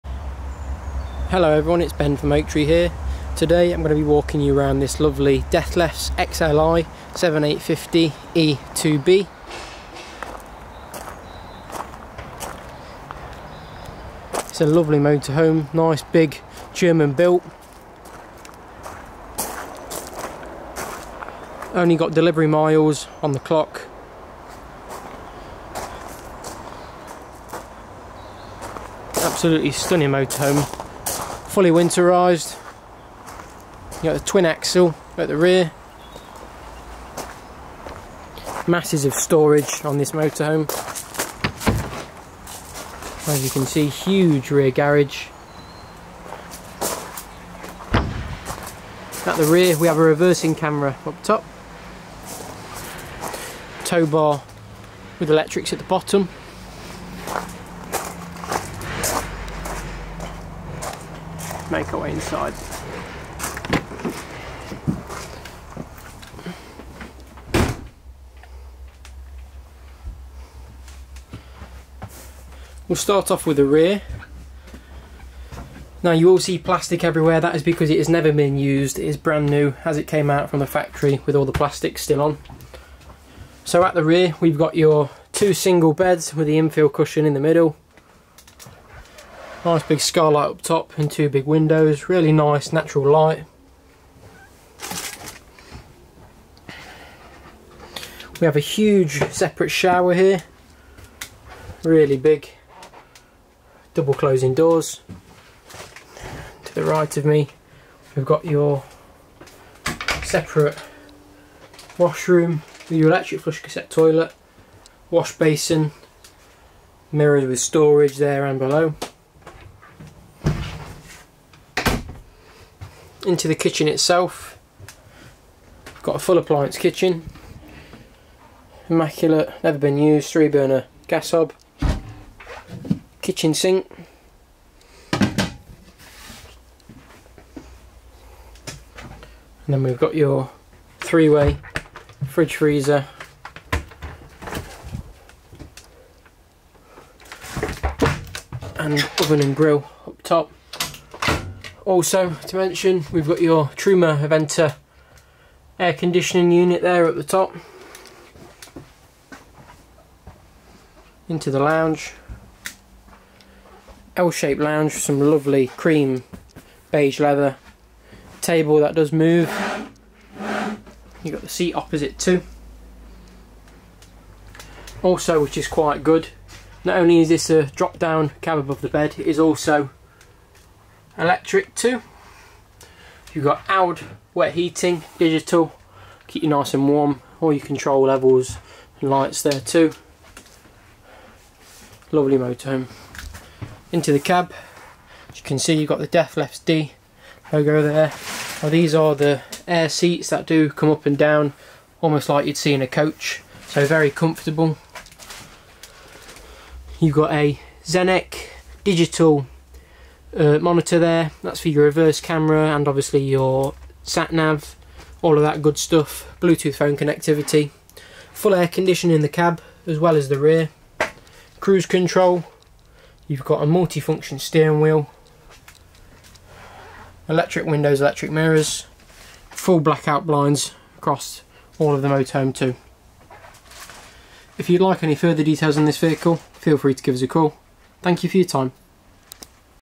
Dethleffs Globetrotter XLI 7850 2 EB Video Walkaround
Ben Matthews from Oaktree Motorhomes takes you for a walkaround of this Dethleffs Globetrotter XLI 7850 2